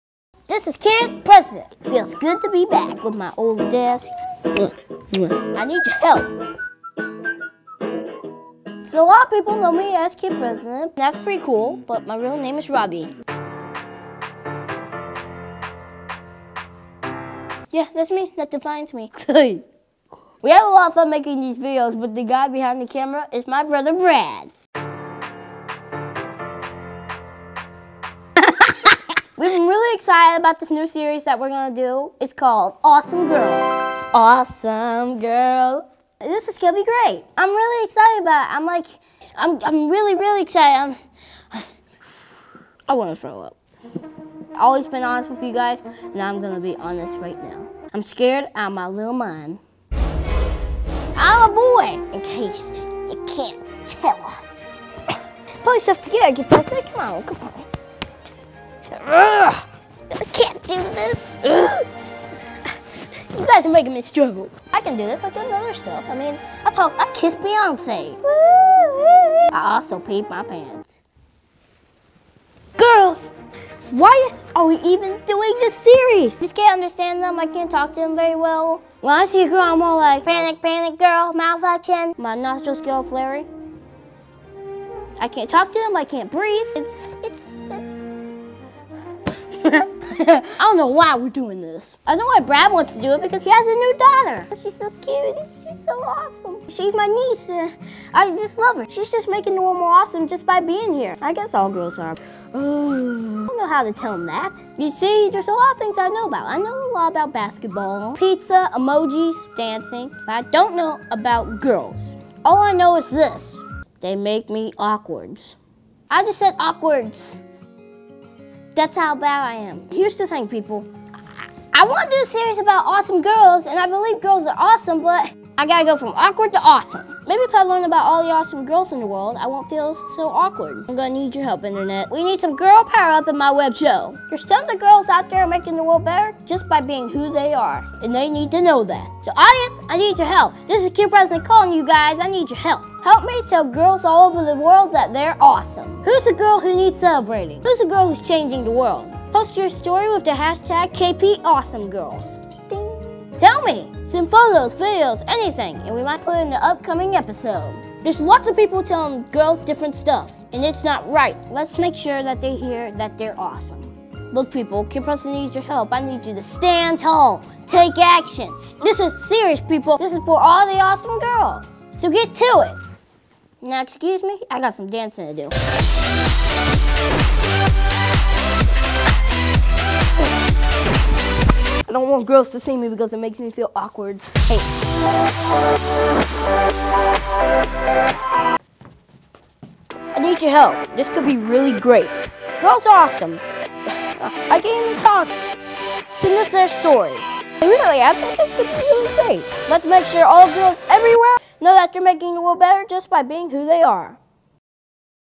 AWESOME GIRLS! | Kid President
Kid President, along with some help and pressure from his brother Brad, is setting out on a mission to let all girls across the world know they are awesome! Do you know a girl who is awesome? Tell her! Also - tell us! Share any photos and videos with the tag KPAwesomeGirls and it might be included in an upcoming episode!

More to come. It’s time to celebrate girls.